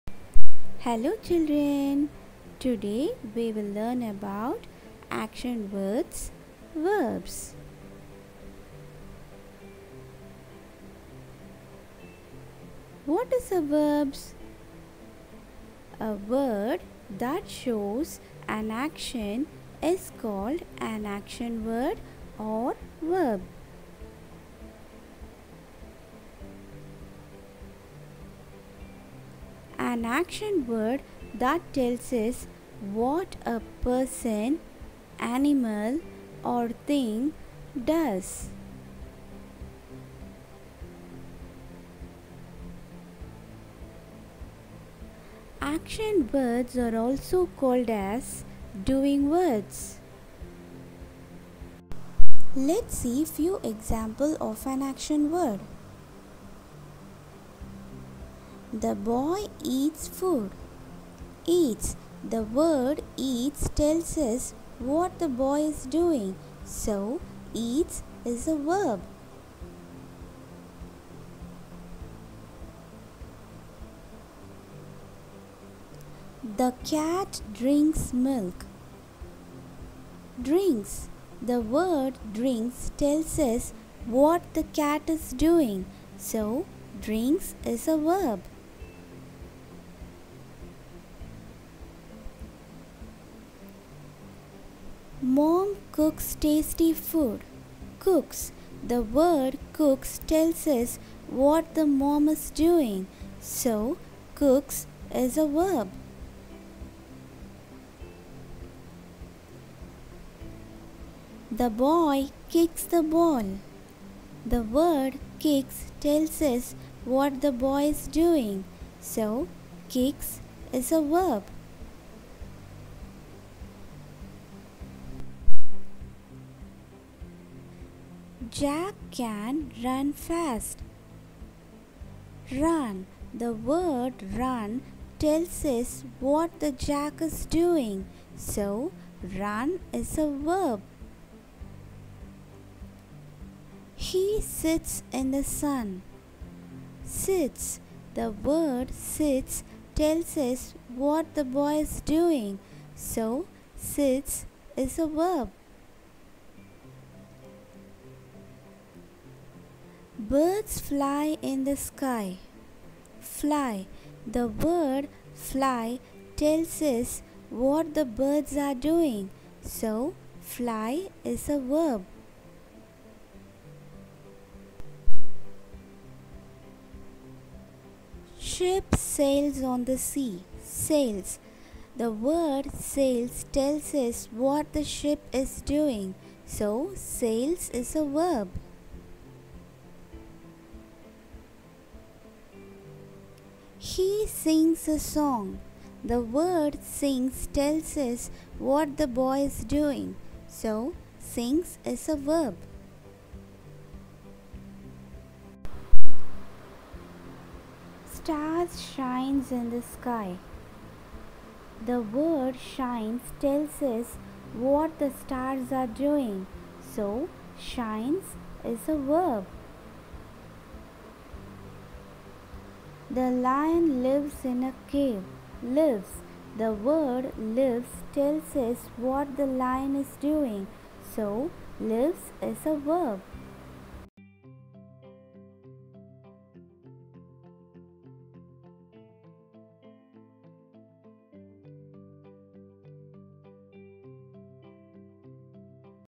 Verbs | Class 1 | CBSE
This video helps the kids to understand the basic concept of verbs also called an action words or doing words.